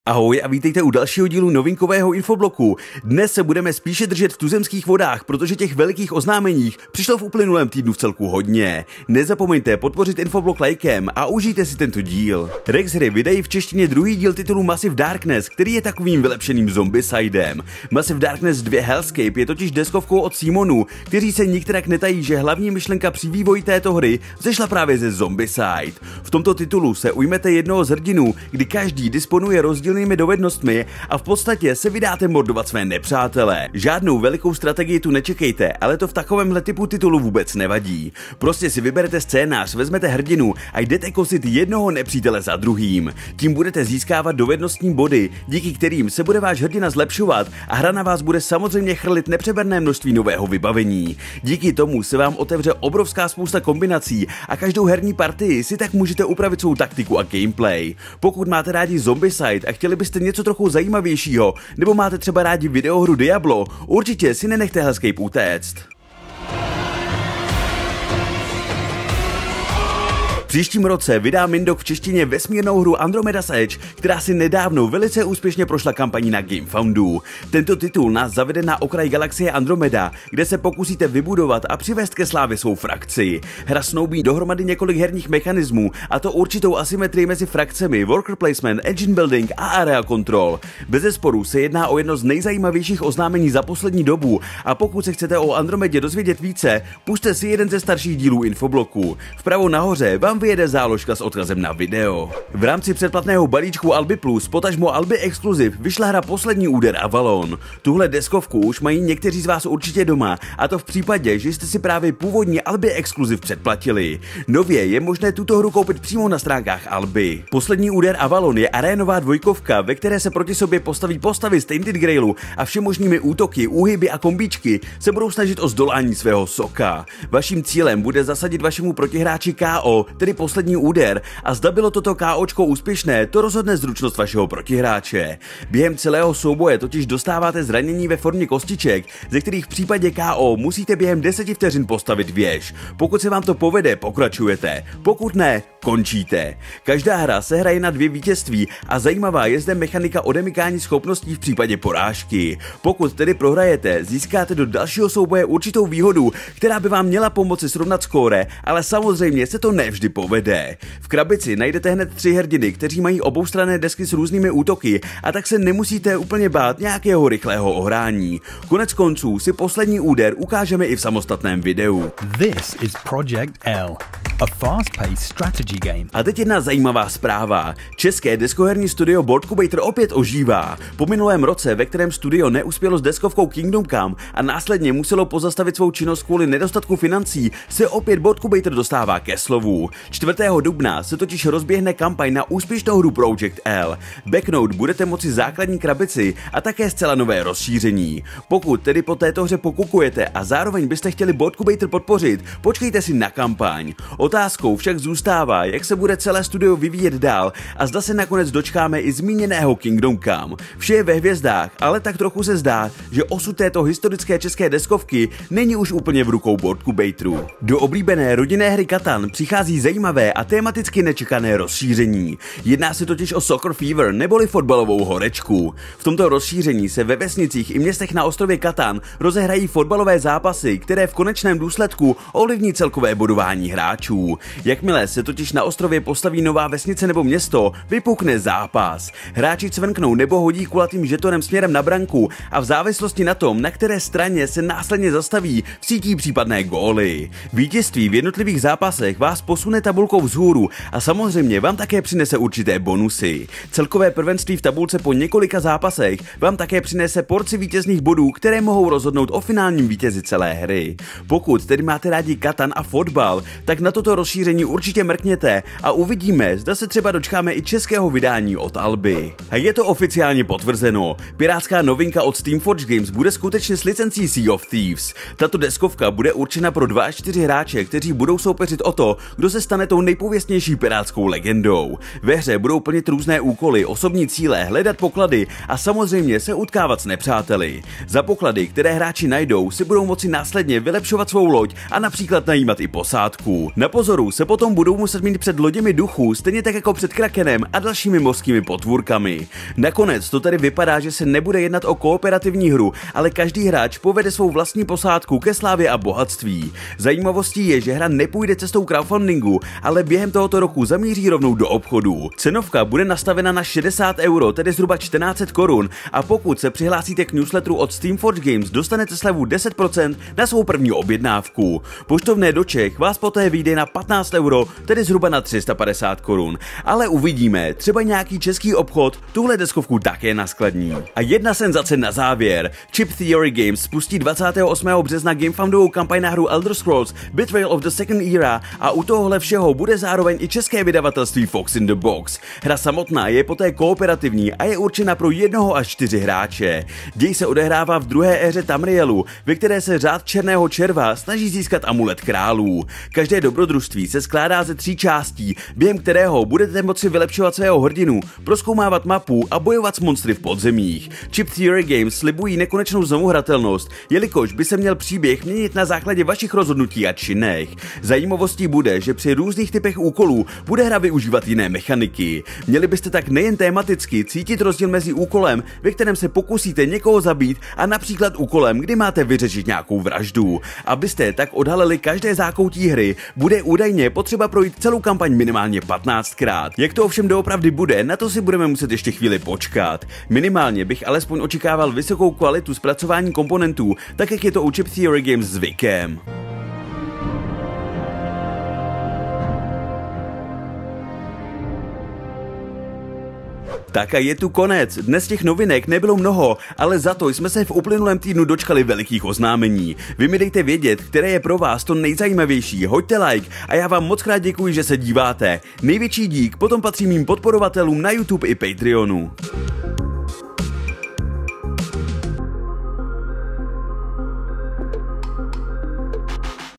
Novinkový Infoblok - Andromeda's Edge od Mindoku a Elder Scrolls od Chip Theory v češtině 🗞️
Novinkový Infoblok Vám přináší shrnutí toho nejzajímavějšího z deskoherního světa za uplynulý týden.

💸Podpora kanálu je možná formou členství zde na Youtube nebo na Patreonu

00:00 - Úvod
00:12 - Massive Darkness 2: Hellscape vyjde díky Rexhrám v češtině
01:10 - Vesmírnou hru Andromeda's Edge vydá Mindok
01:42 - Arénová dvojkovka Poslední Úder: Avalon je skladem
02:51 - České studio Boardcubator se vrací zpět na scénu
03:37 - Do Catanu přichází fotbalové rozšíření
04:29 - Pirátská novinka od SFG bude skutečně Sea of Thieves
05:30 - Elder Scrolls od Chip Theory Games vyjde díky Fox in the Box v češtině
06:43 - Závěr a poděkování 👑